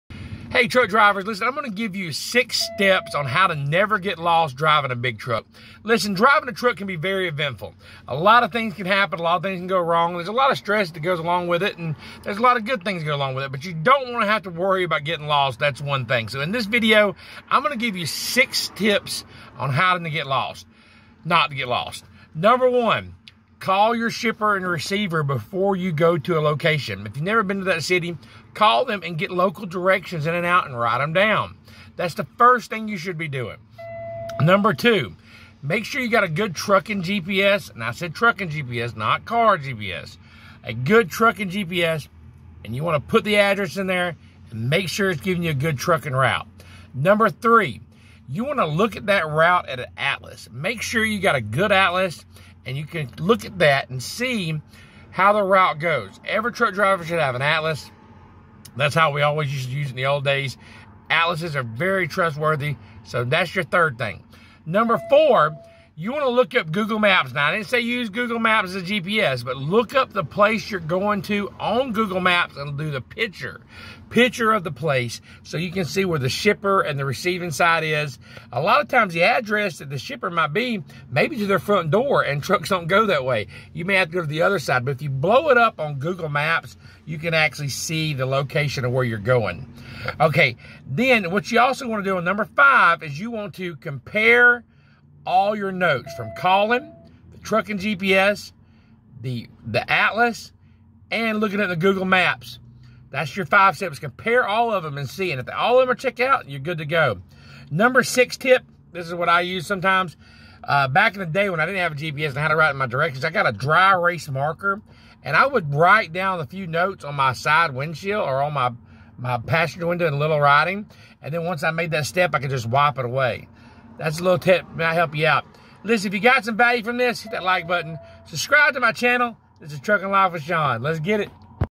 6 tips every truck driver MUST know so they never get lost in a big 🚚 truck
six tips after truck driver must know not to get lost in a big truck getting lost driving a commercial vehicle can be very stressful take these steps to ensure that never happens truckdriver trucking cdl otr @MuthaTrucker @SmartTrucking @BoneheadTruckers @truckinglifewithshawn